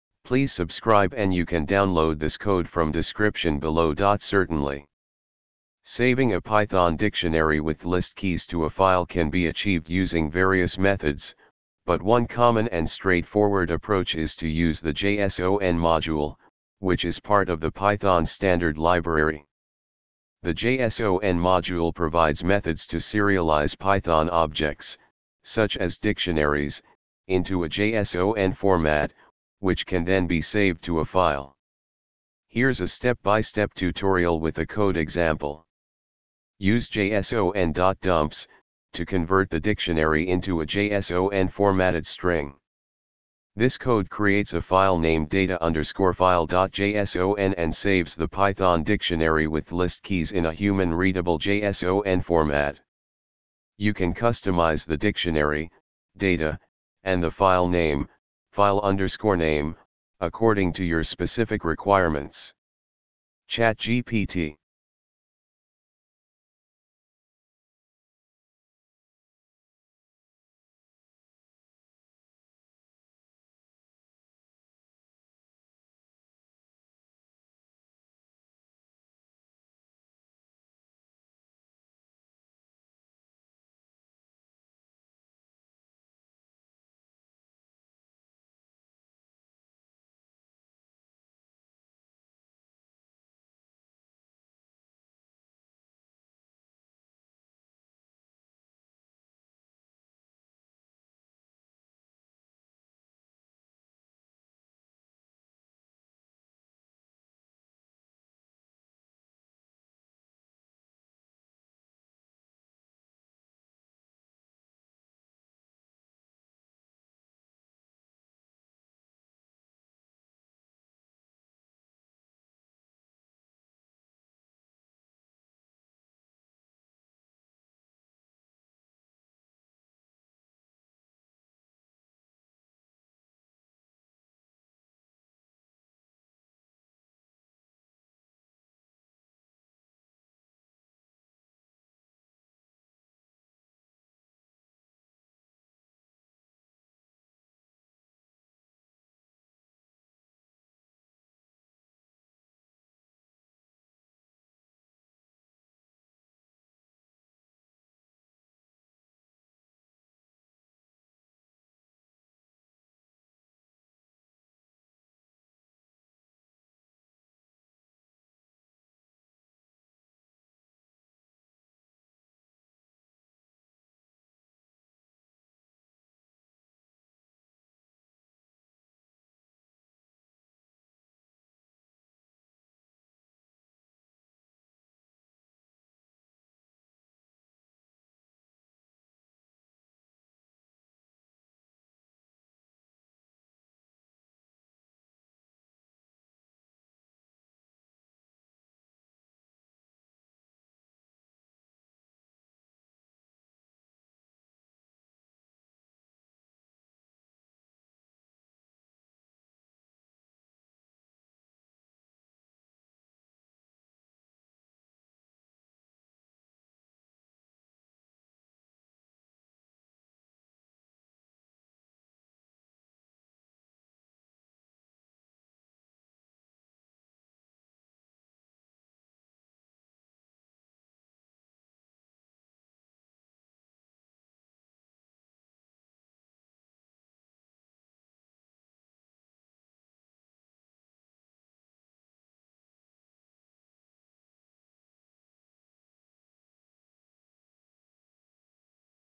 How to save a python dictionary with list keys to a file
Certainly! Saving a Python dictionary with list keys to a file can be achieved using various methods, but one common and straightforward approach is to use the json module, which is part of the Python standard library. The json module provides methods to serialize Python objects, such as dictionaries, into a JSON format, which can then be saved to a file.
Here's a step-by-step tutorial with a code example:
Use json.dumps() to convert the dictionary into a JSON-formatted string.
This code creates a file named data_file.json and saves the Python dictionary with list keys in a human-readable JSON format. You can customize the dictionary (data) and the file name (file_name) according to your specific requirements.
ChatGPT